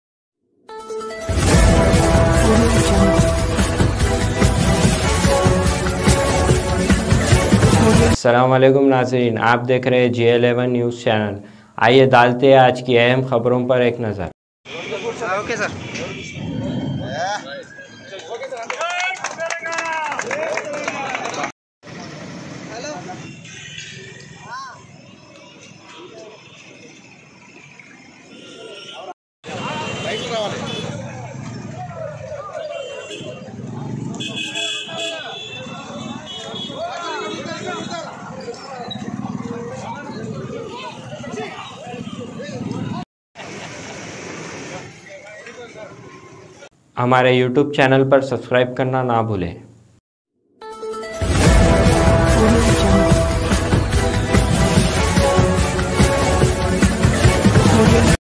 MLA C.LakshmaReddy Inaugurated CenterLights From the Jadcherla Police Station To SignalGadda|J11News
మహబూబ్ నగర్ జిల్లా
జడ్చర్ల  పోలీస్ స్టేషన్ నుంచి సిగ్నల్ గడ వరకు ఏర్పాటుచేసిన సెంటర్ లైటింగ్ సిస్టమ్ ను ప్రారంభించిన ఎమ్మెల్యే లక్ష్మారెడ్డి